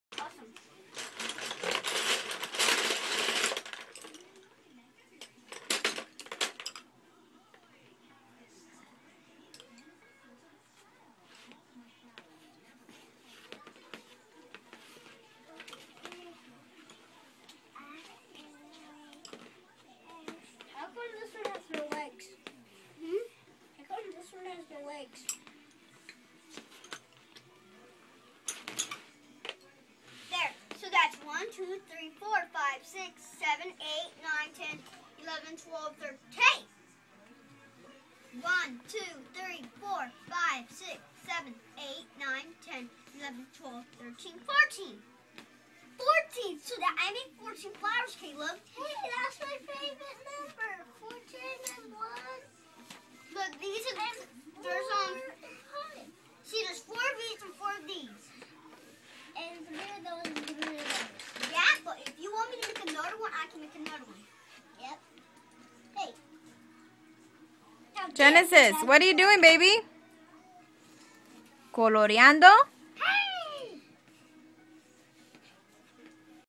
My babies coloring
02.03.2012
They were coloring roses for Cedibel their sister in Panama